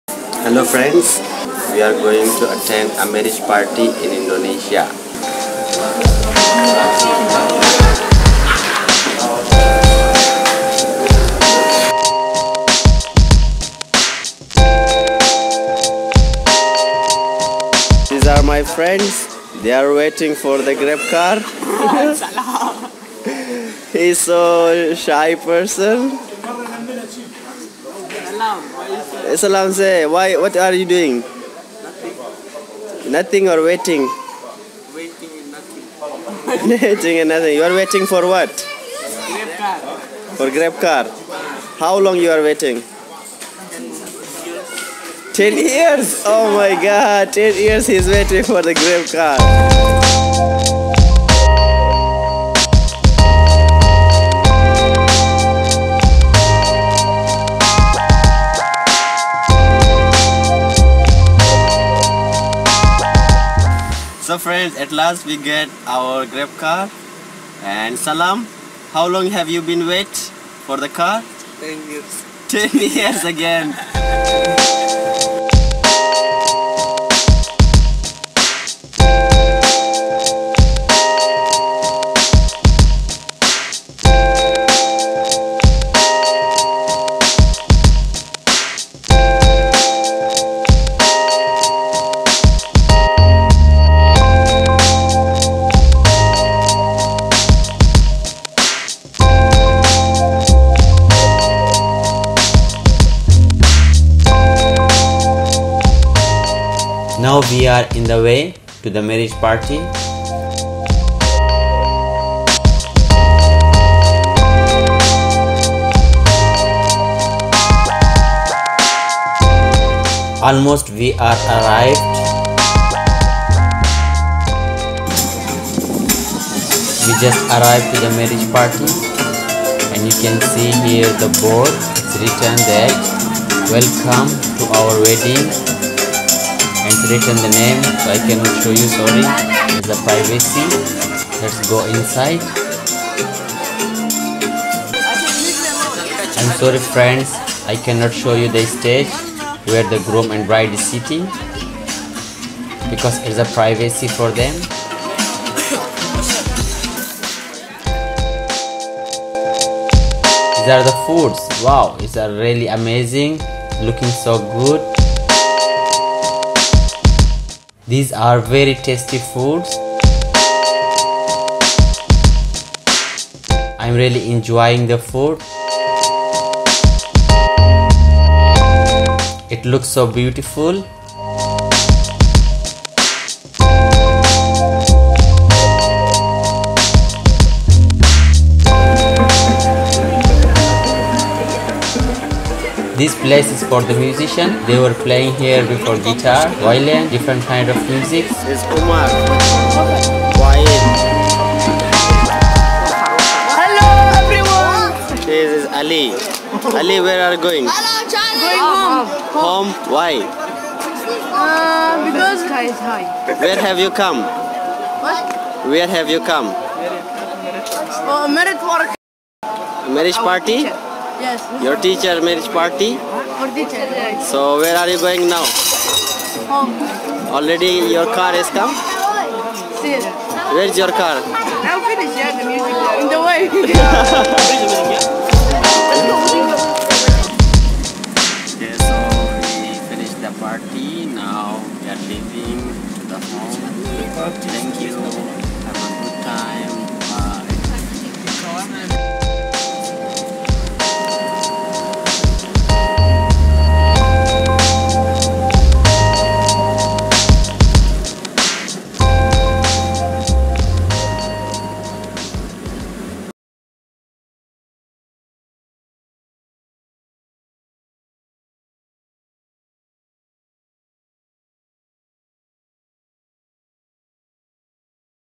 Vlog#03 Indonesian Marriage Party (with Hazara Boy)
This is my 3rd VLog and I am invited in a INDONESIAN Marriage Party .